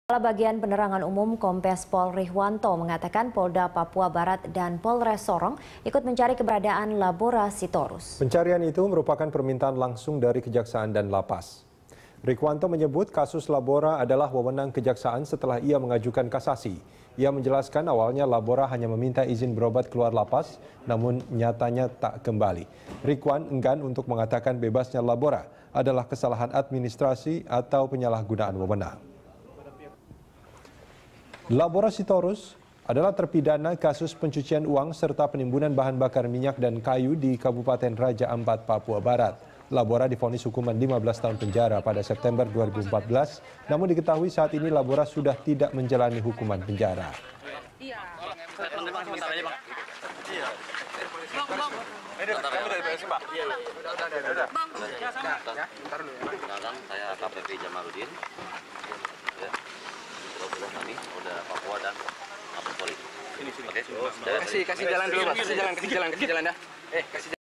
Polisi Lacak Keberadan Labora Sitorus
Jajaran Kepolisian Daerah Papua Barat dan Kepolisian Resor Sorong ikut mencari keberadaan Labora Sitorus. Pencarian ini merupakan permintaan langsung dari kejaksaan dan lapas.

Facebook.com/BeritaSatuTV
Youtube.com/BeritaSatu